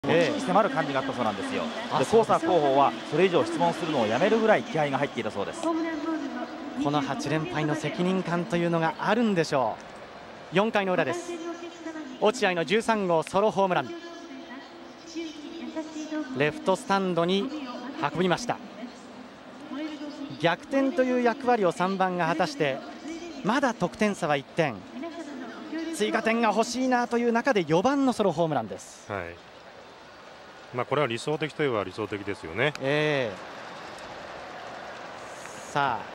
【右肘の送り方に注目】落合博満 シーズン13号ホームラン（通算465本目）1994年9月6日
対 田辺学（横浜）左方向
江川卓

⚾️落合博満（40歳）
（ロッテオリオンズ〜中日ドラゴンズ〜読売ジャイアンツ（巨人）〜日本ハムファイターズ）
1994年シーズン成績：129試合 .280 15本 68打点

右肘の送り方を見ると、外角寄りのボールに照準があったかもしれない。
インパクトまでにうまくアジャスト。右肘を体に引き付けた後は
大きくフォロースルーし飛距離を稼いだ。バットコントロールの名手健在と言ったところか。
右腕が強いことと、足を踏み込んでも上半身が突っ込まず、体幹軸がうまく回転している。